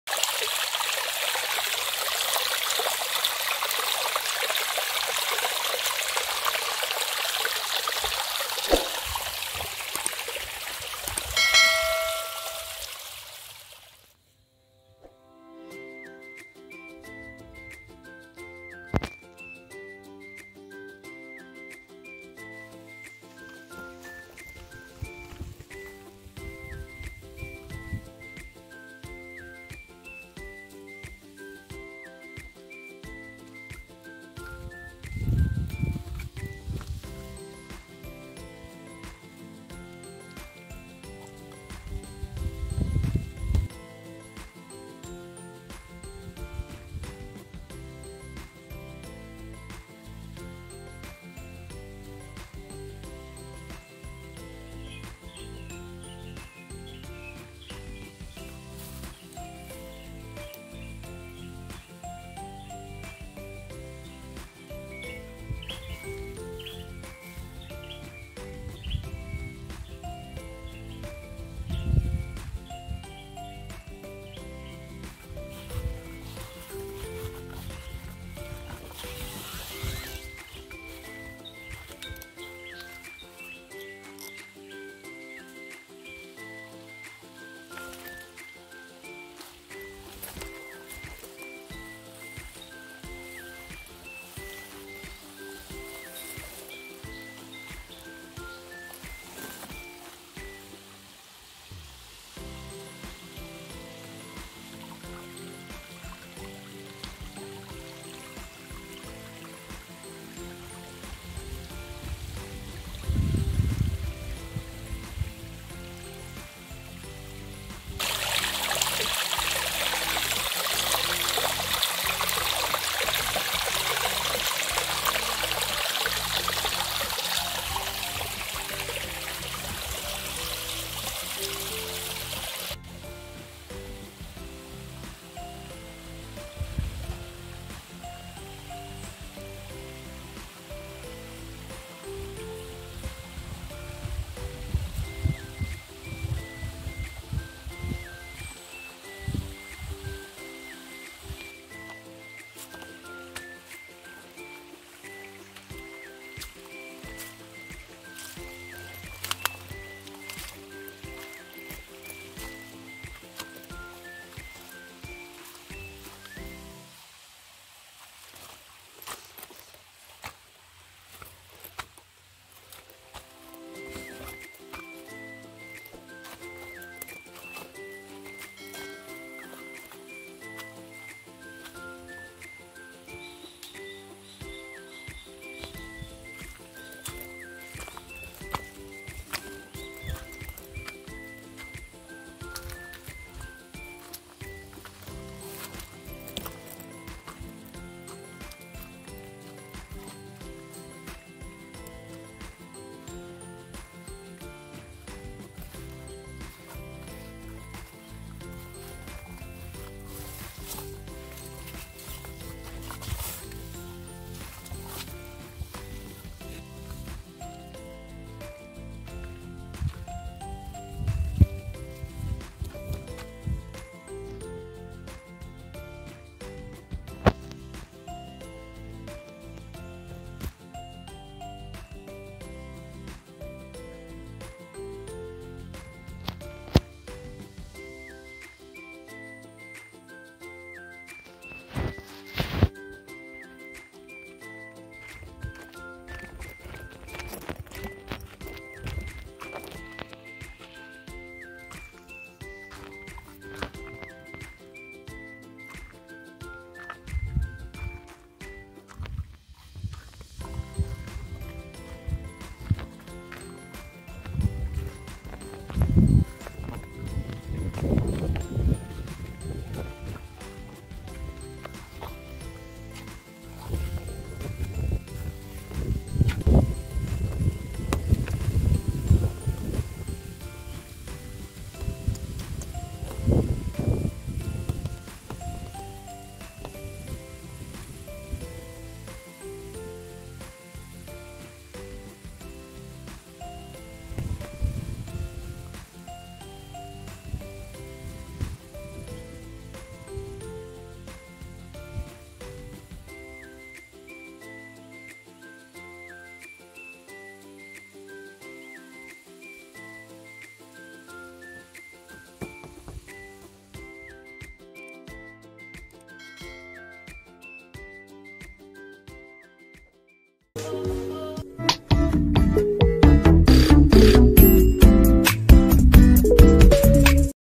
SARAP SA PILING, HANGIN NA NAPAKASARIWA, MAPAKIMBOT KA TALAGA ‼️🤣
A Very nice, clean, and green natural view. A freshwater and air is only here in our province come and witness! The natural beauty of view. Tambayan. The Adventure.